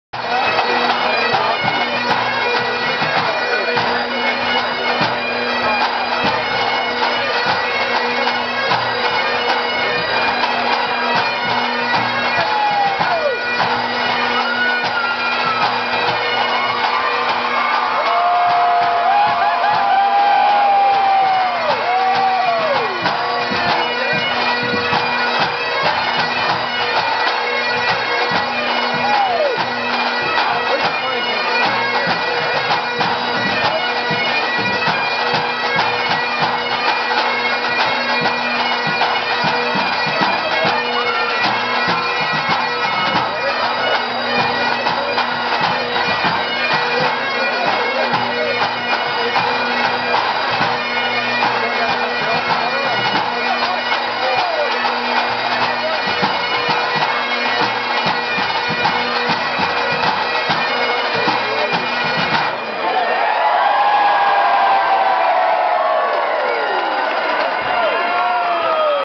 Peel Police at Maxville Beer tent - celebration!
The Peel Police celebrate their Grade One win at Maxville.